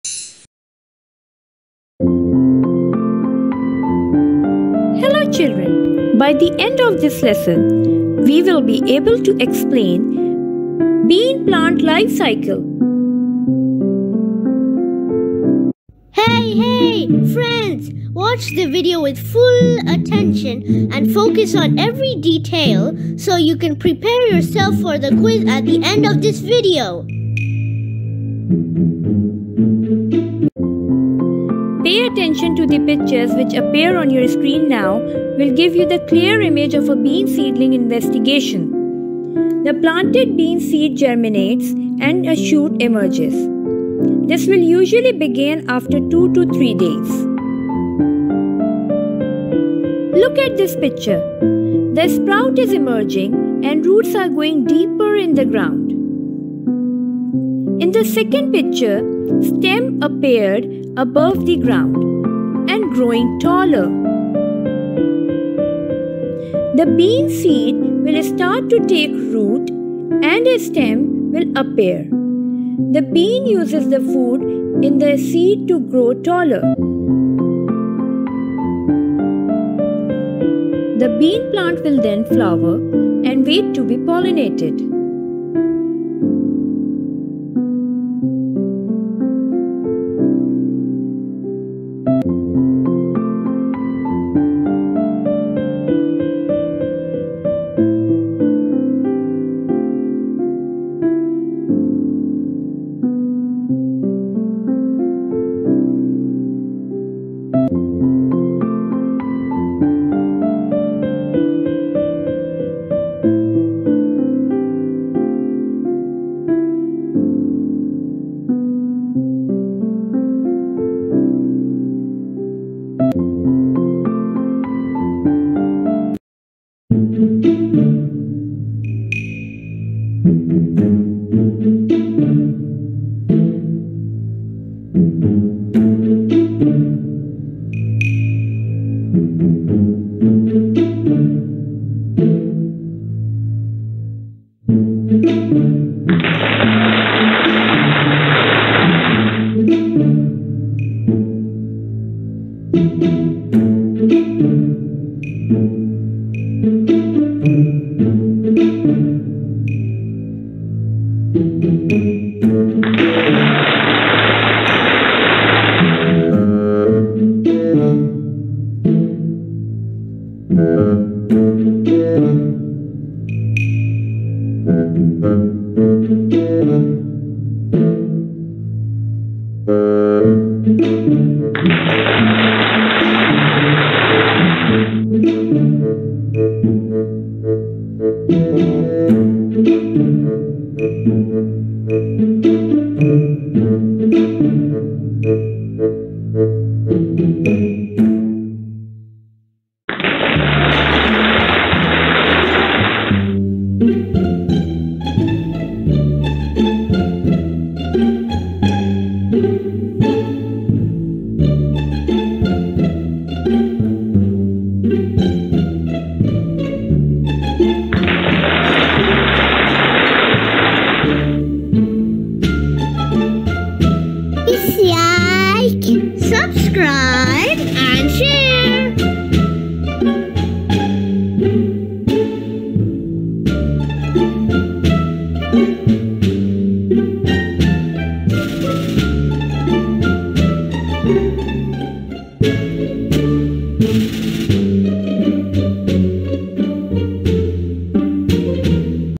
Bean Plant Life Cycle
Learn about bean plant life cycle@funlearnzoneFLZ
The planted Bean seed germinates and a shoot emerges. This will usually begin after 2-3 days. The bean seed will start to take root and a stem will appear. The bean uses the food in the seed to grow taller. The bean plant will then flower and wait to be pollinated.